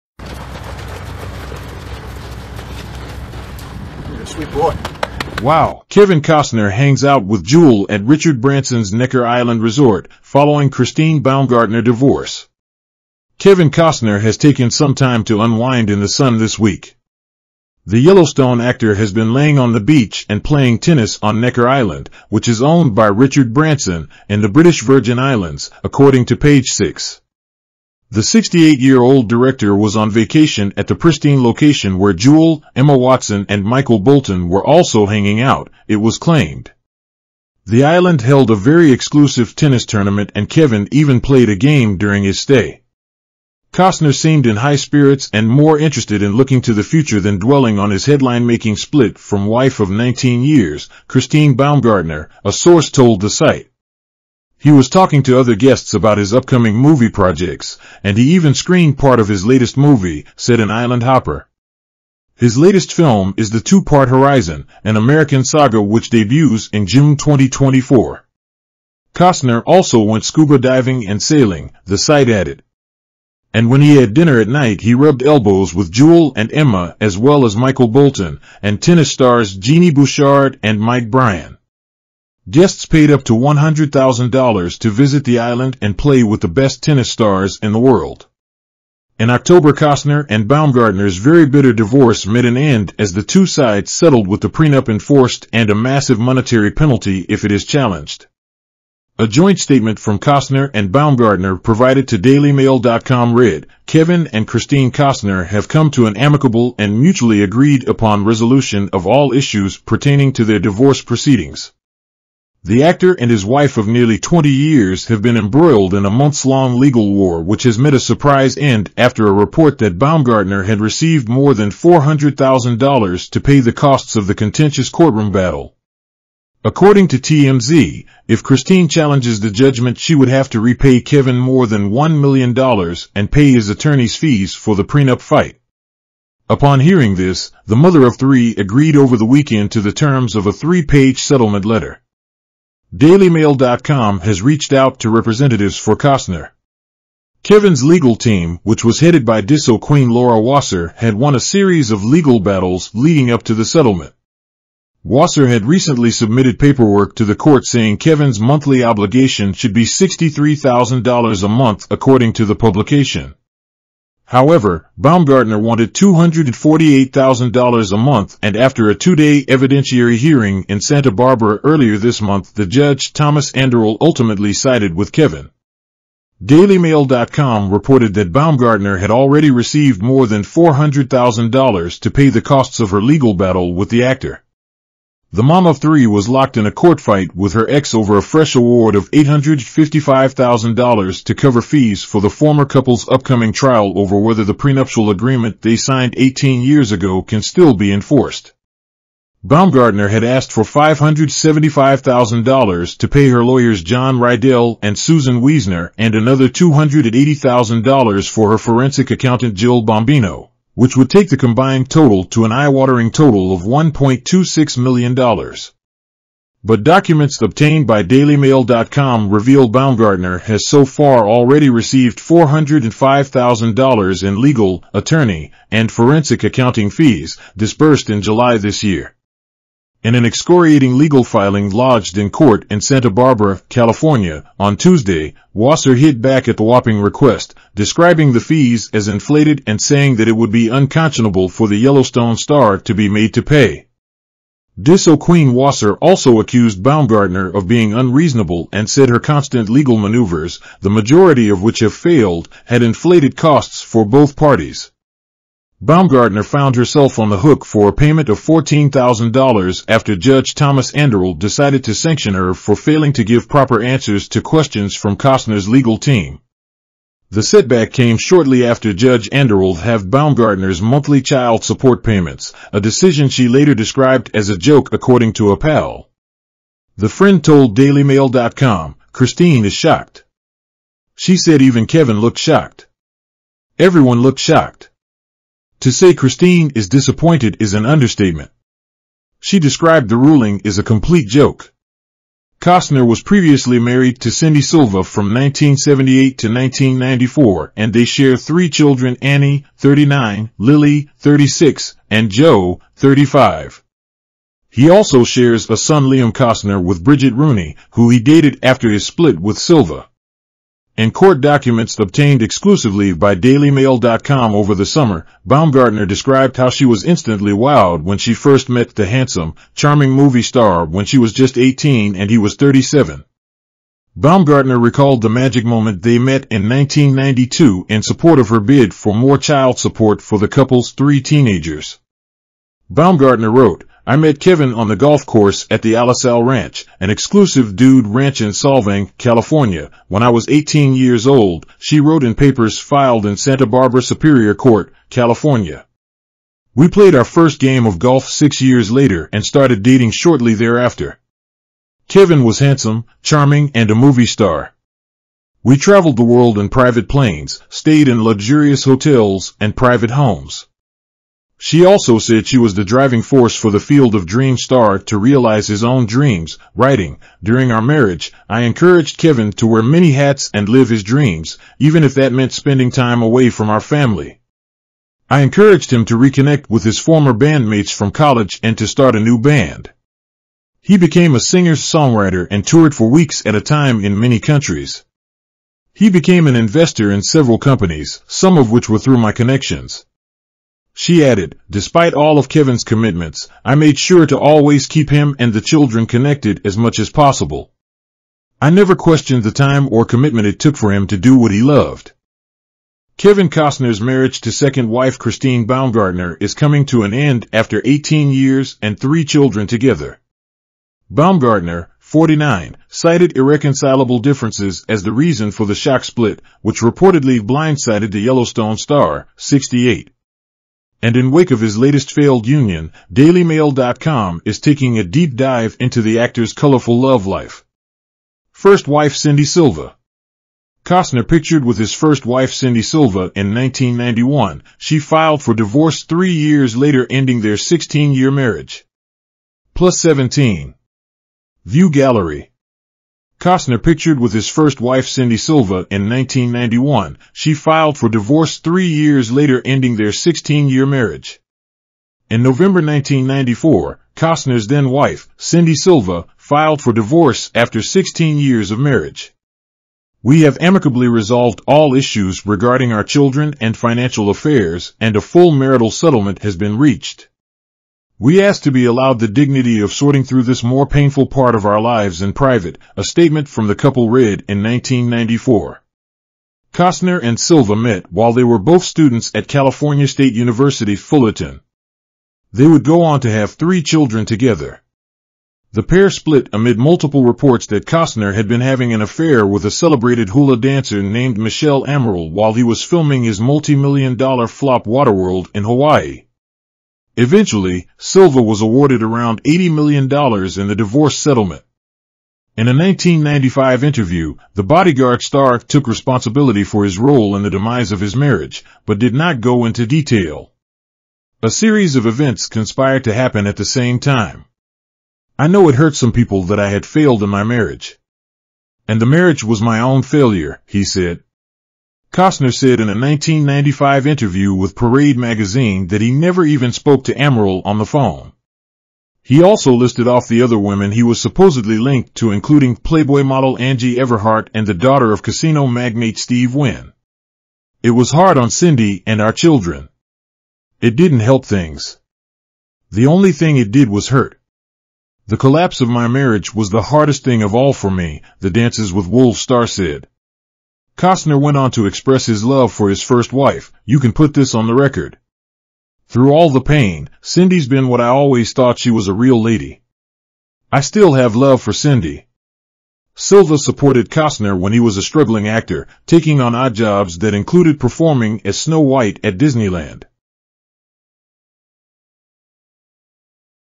Wow! Kevin Costner 'hangs out with Jewel' at Richard Branson's Necker Island resort following Christ
Wow! Kevin Costner 'hangs out with Jewel' at Richard Branson's Necker Island resort... following Christine Baumgartner divorce